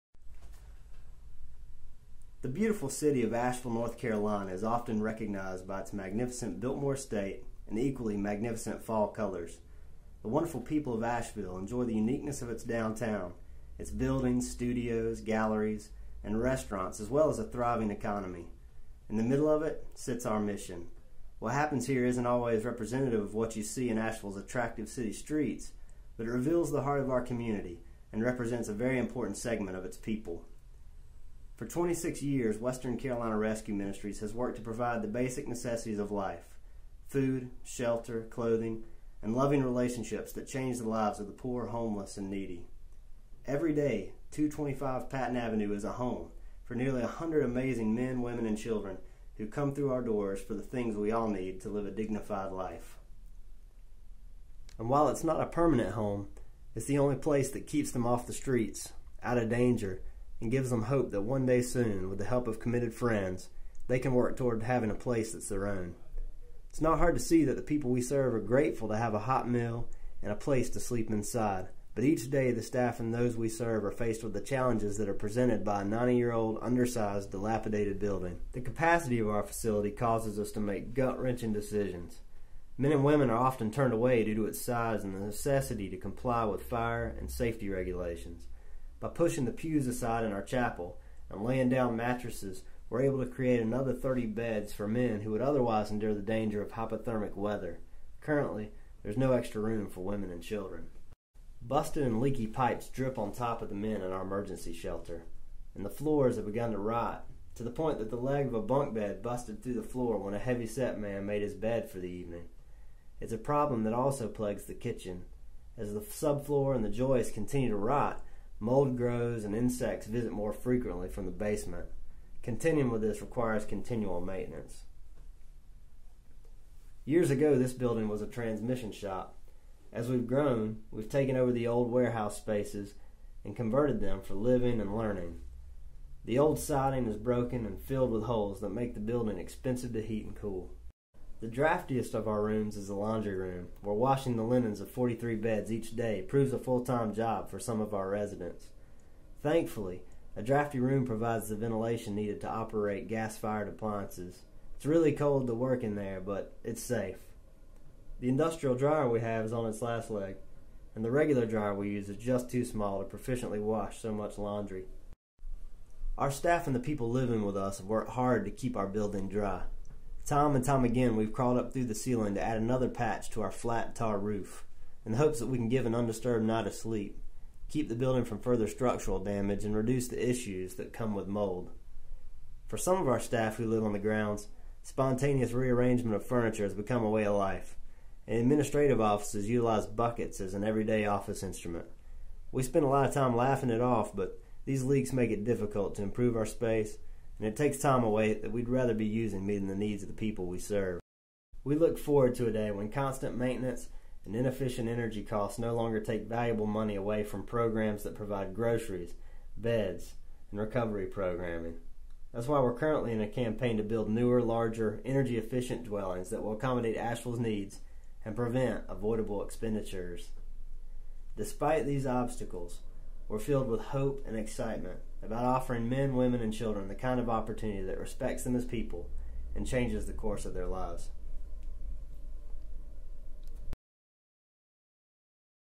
Where the Mission Lives
A close look at the place where Western Carolina Rescue Ministries works to care for the poor, homeless, and broken in Asheville, NC.